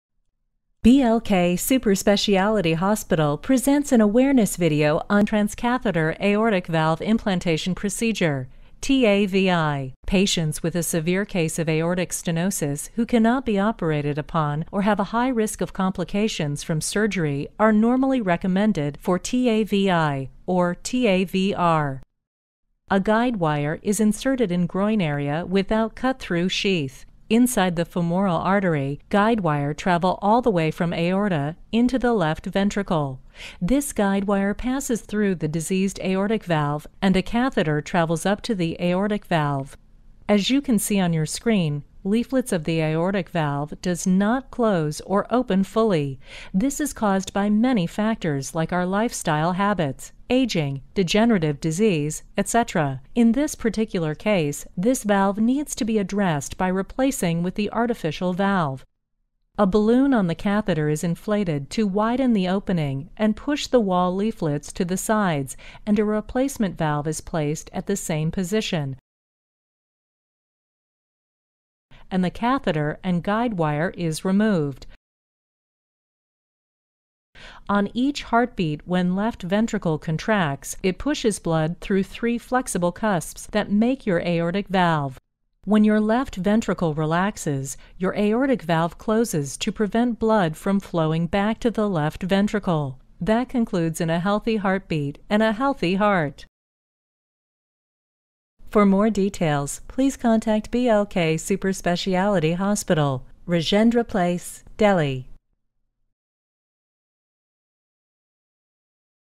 MedAni BLK TAVI
Patient education on Tarnscatheter Aortic Valve Replacement procedure